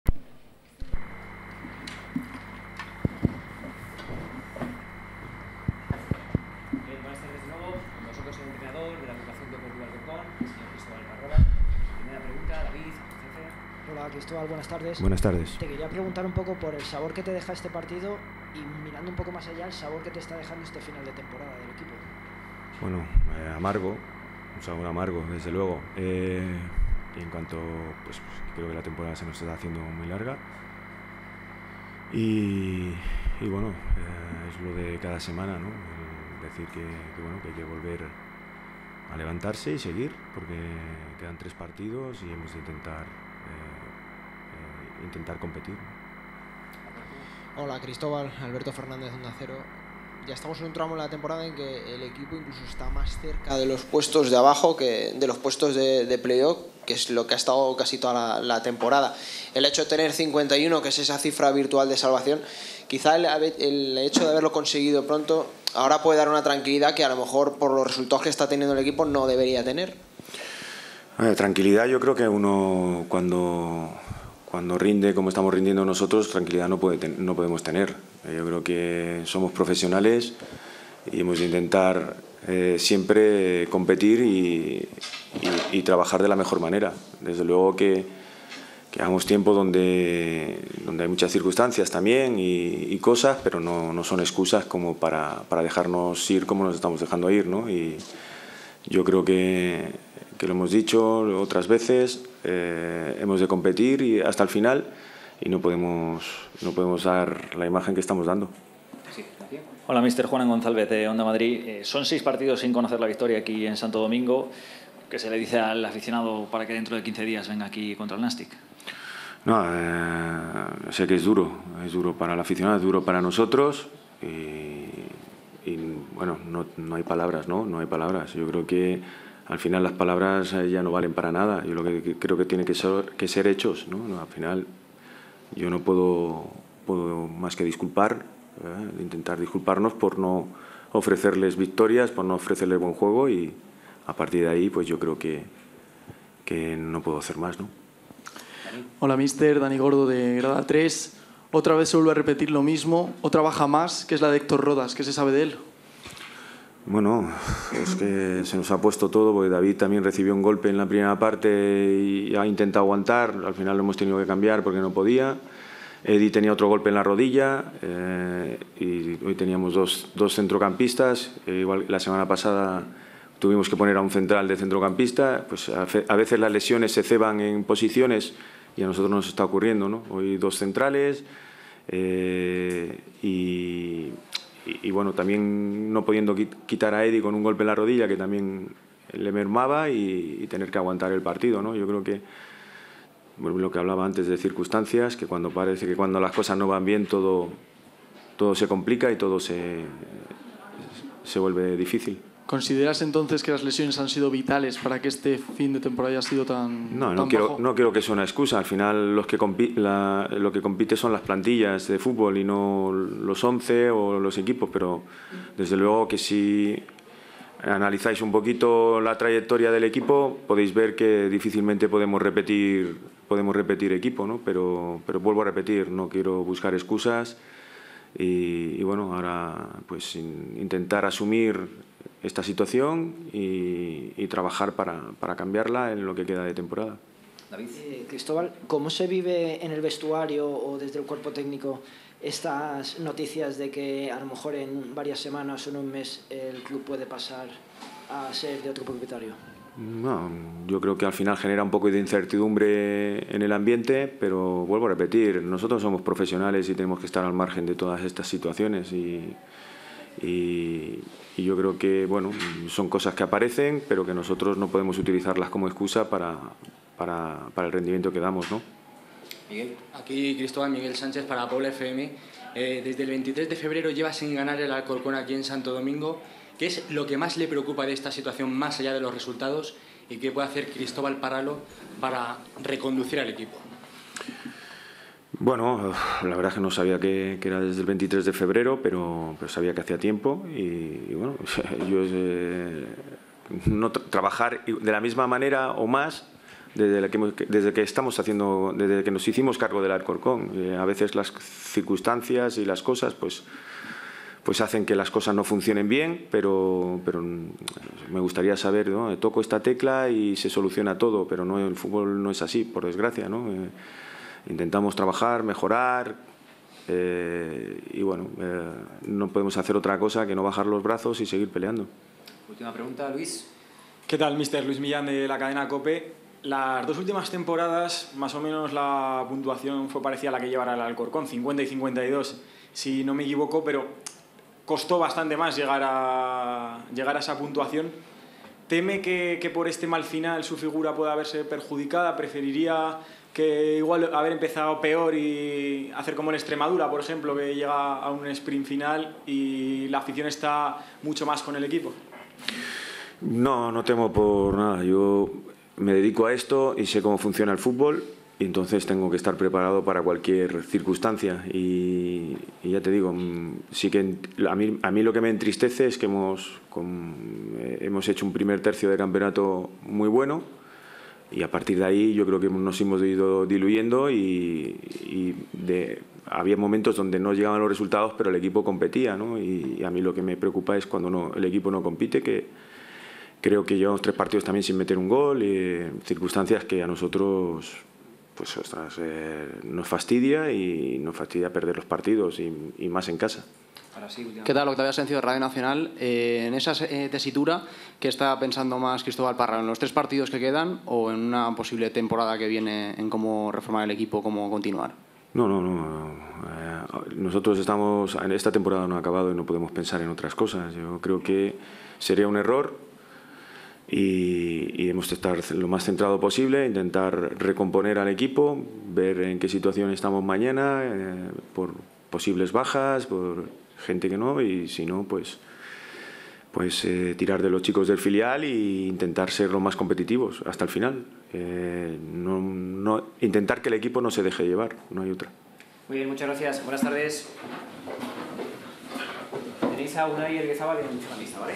Rueda de prensa de Cristóbal Parralo tras el AD Alcorcón vs Extremadura UD (0-1)
Rueda de prensa de Cristóbal Parralo tras el AD Alcorcón vs Extremadura UD (0-1) J39 LaLiga 1|2|3 2018/2019